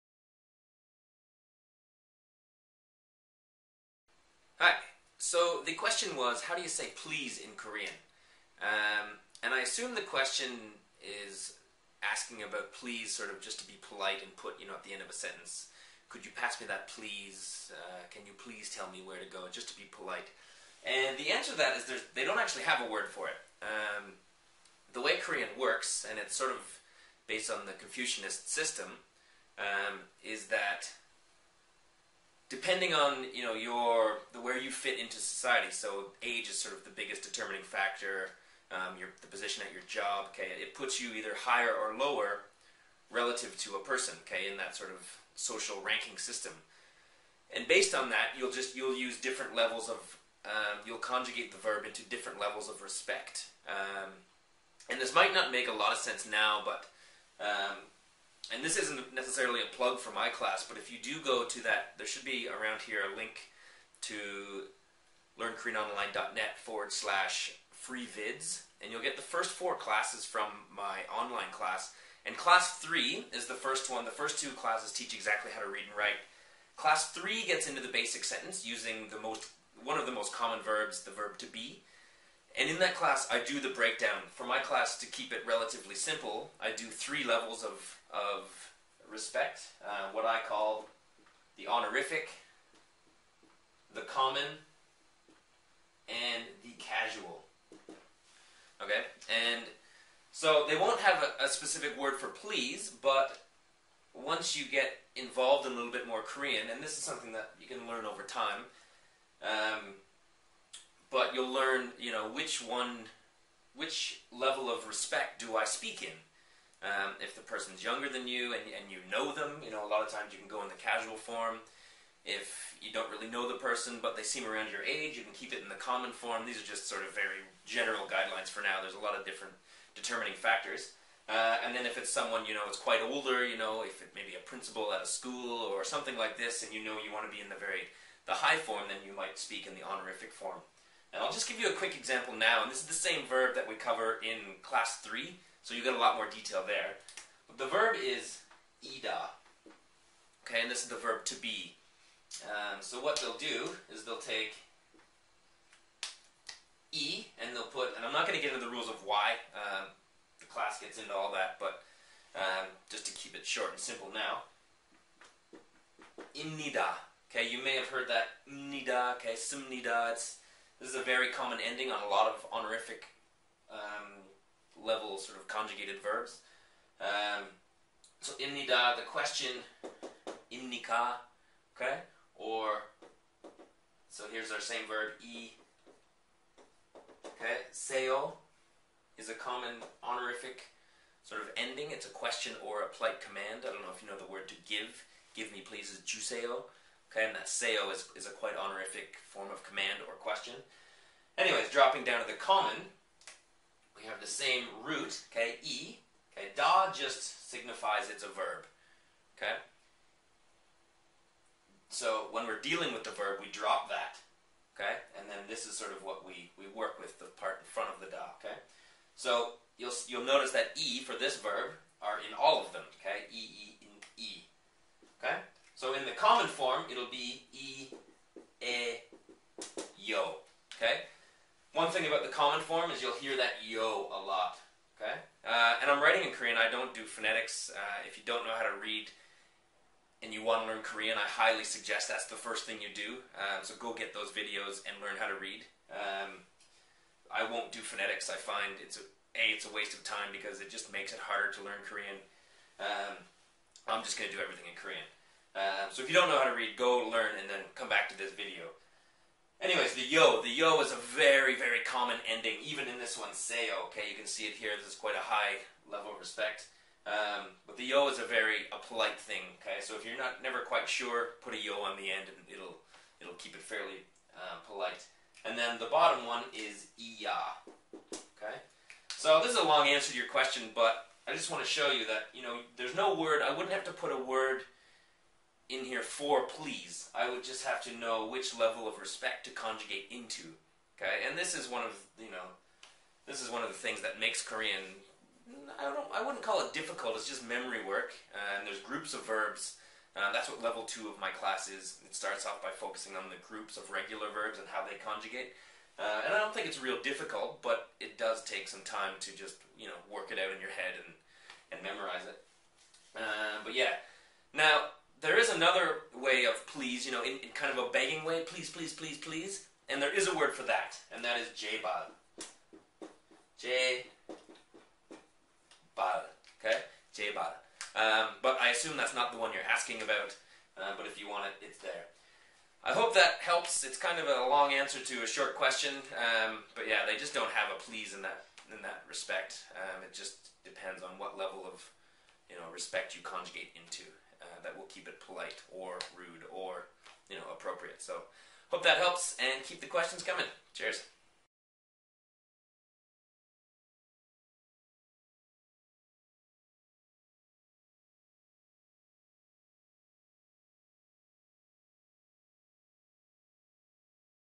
'How-to Korean' Video #2 - How do you say PLEASE in Korean
This is video #2 in the 'How-to Korean' series series teaching basic Korean starting with how to read and write, followed by teaching basic phrases and other explanations in simple, easy-to-understand English.  To get 4 hours of vids teaching exactly how to read and write and then getting into the basic sentence, check out the link above.  Cheers!